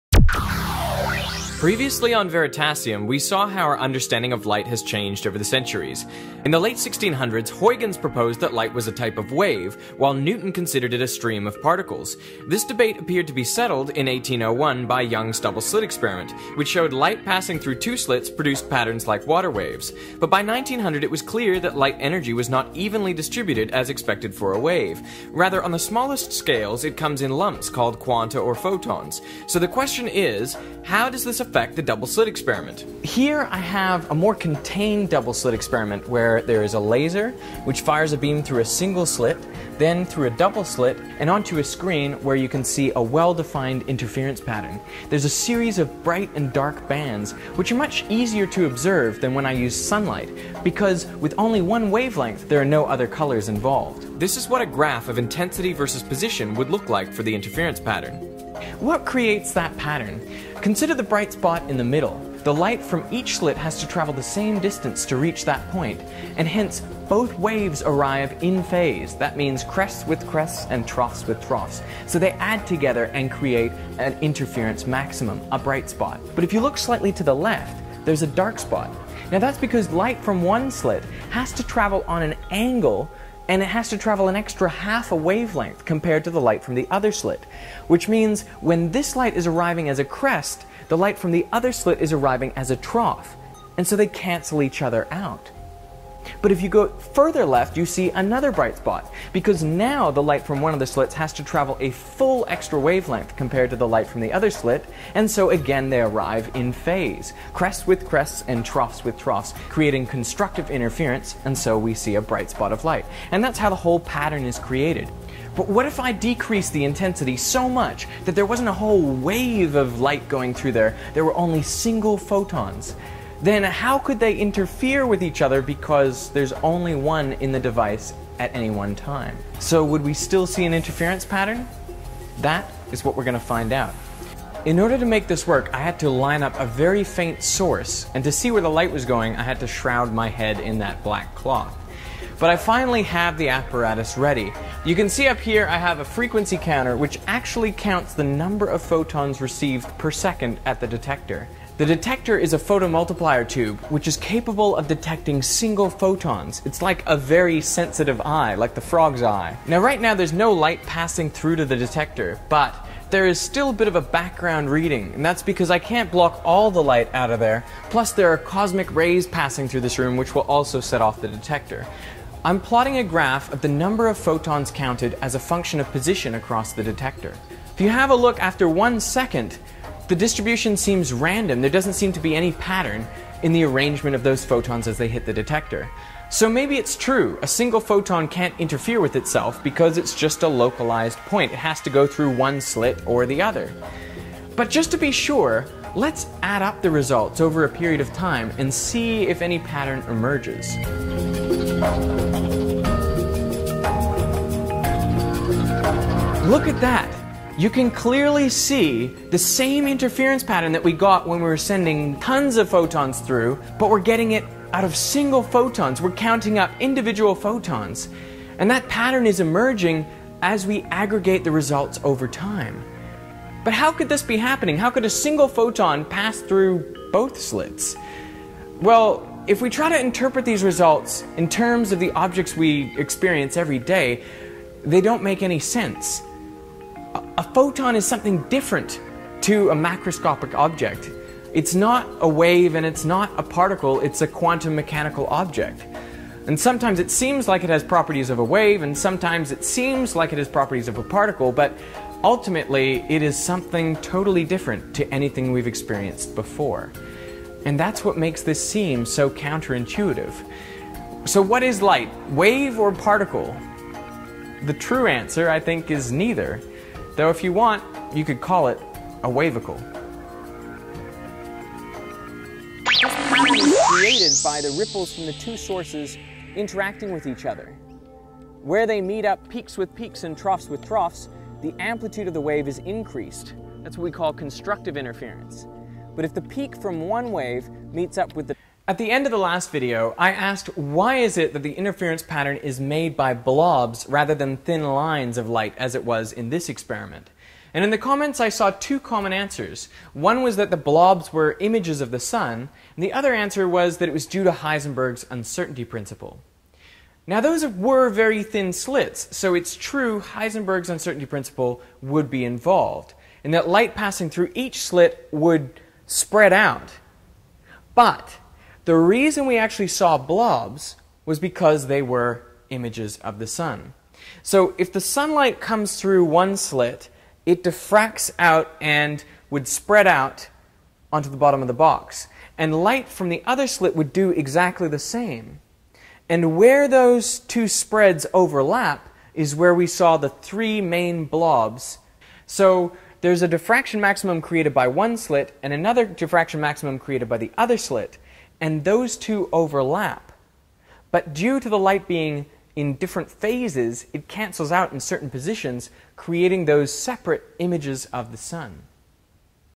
Single Photon Interference | Photons of Light
What happens when single photons of light pass through a double slit and are detected by a photomultiplier tube?

 In 1801 Thomas Young seemed to settle a long-running debate about the nature of light with his double slit experiment.

He demonstrated that light passing through two slits creates patterns like water waves, with the implication that it must be a wave phenomenon.

However, experimental results in the early 1900s found that light energy is not smoothly distributed as in a classical wave, rather it comes in discrete packets, called quanta and later photons. These are indivisible particles of light.

So what would happen if individual photons passed through a double slit? Would they make a pattern like waves or like particles?